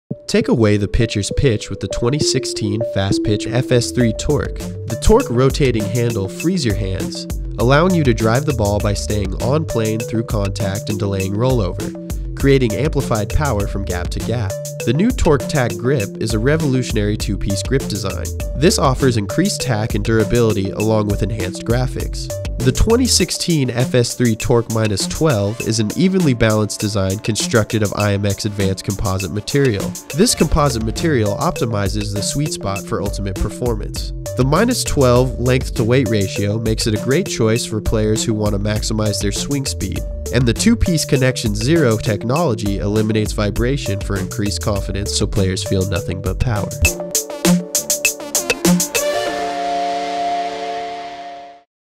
Easton 2016 FS3 Torq Fastpitch Bat
Fastpitch players at every level are discovering the amplified power that comes from Easton's revolutionary Torq technology. The innovative rotating handle helps hitters stay inside the ball, getting them shorter to the zone and longer through the zone for more squared-up power to all fields. The FS3 Torq -12 is an evenly balanced design constructed of IMX advanced composite material to optimize the sweet spot for ultimate performance. The -12 length-to-weight ratio makes it a great choice for player's who want to maximize their swing speed and two-piece CXN Zero technology eliminates vibration for increased confidence and the true feel of power.